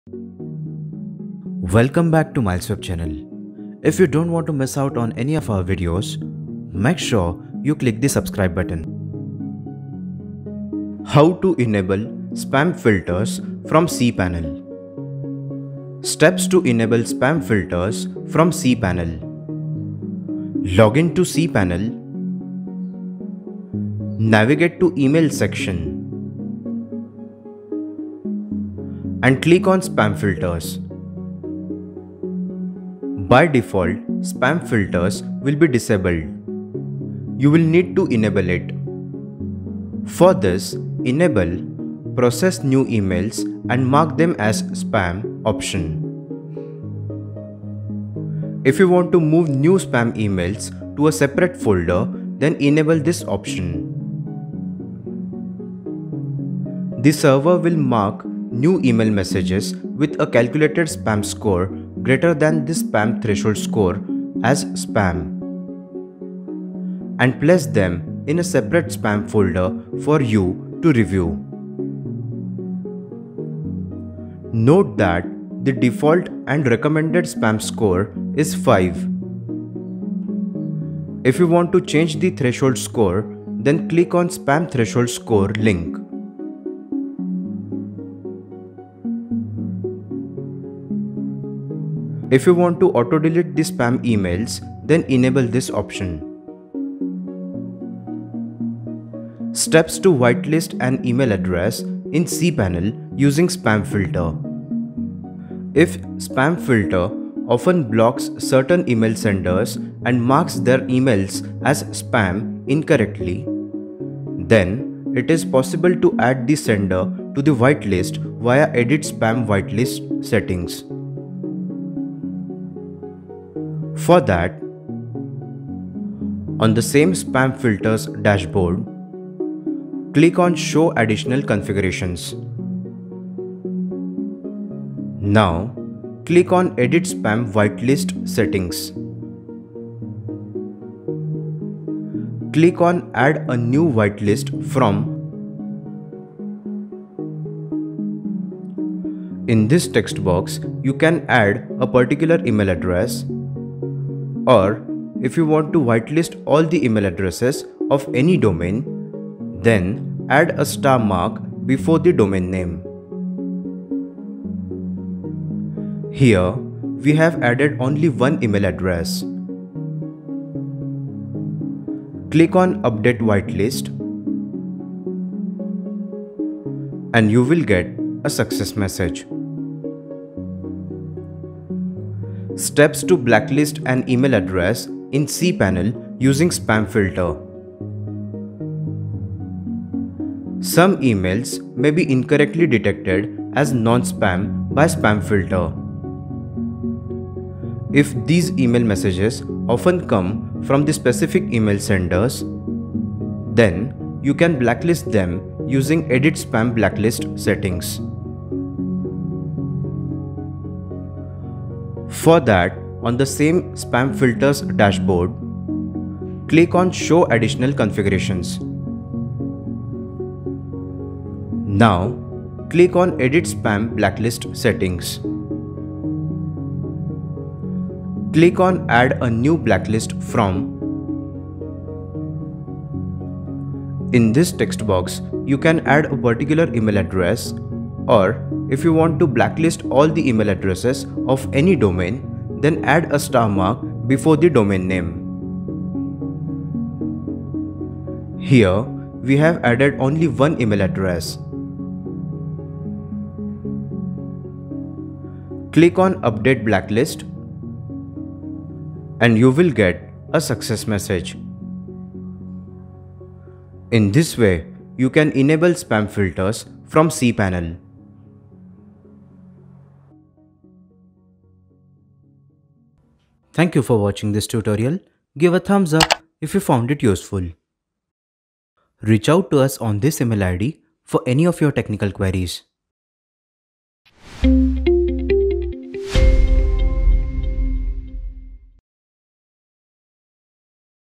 How to Enable Spam Filters from cPanel? | MilesWeb
Here's an in-depth guide on how to enable spam filters from cPanel:
Contents of this video:
0:00 - Intro
0:16 - Steps  to enable spam filters from cPanel
1:39 - Steps to Whitelist an email address in cPanel using Spam Filter
3:03 - Steps to Blacklist an email address in cPanel using Spam Filter